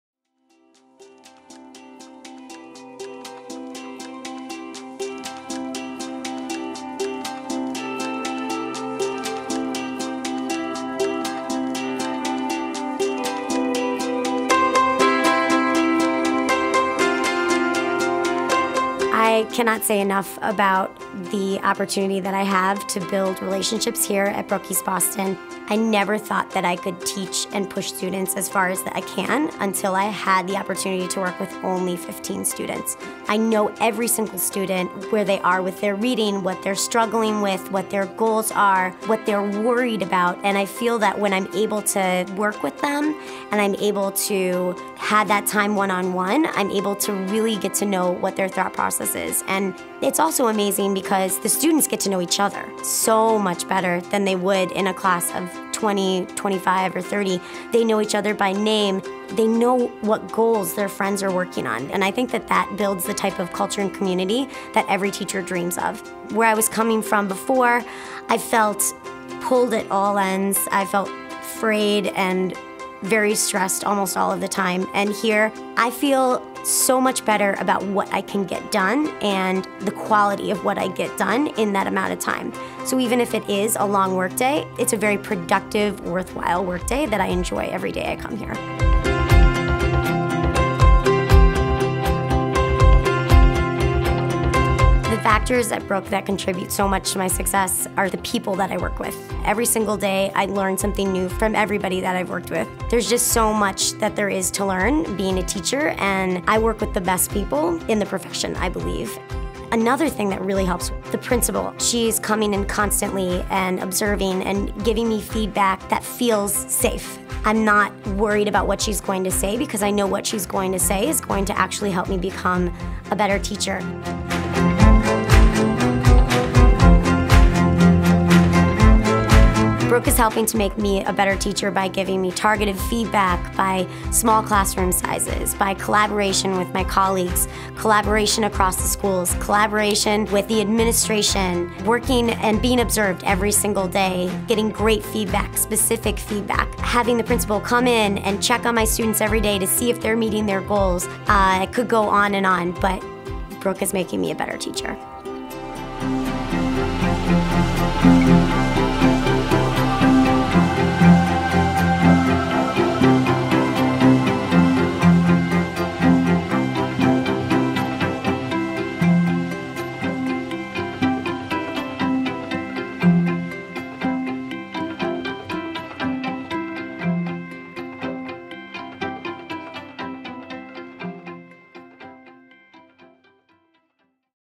BrookeCharterSchools_Teach at Brooke
Katherine Barnes, Brooke East Boston First Grade teacher, discusses why she teaches at Brooke.

Take the next step in your teaching career. Apply to become a Brooke teacher